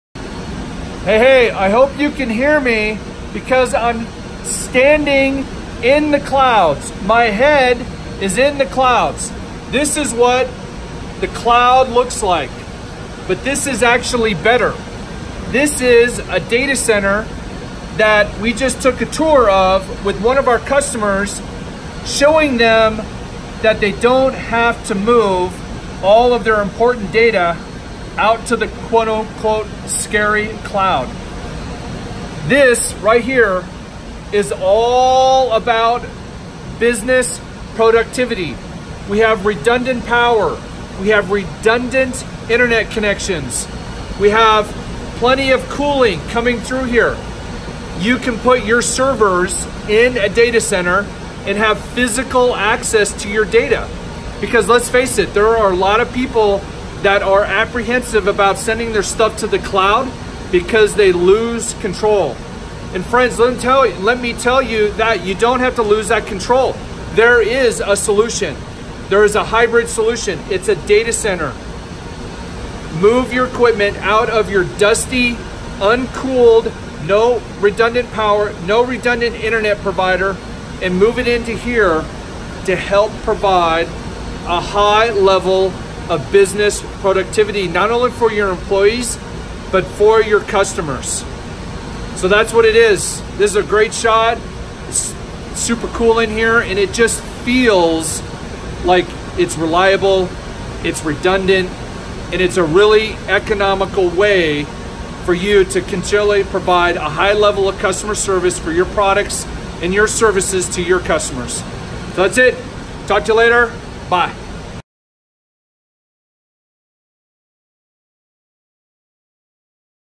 Local or cloud? Get the best of both worlds with the power and network redundancies of a data center
A data center can ensure productivity with power and network redundancies without the loss of physical access to your data. Want to learn more?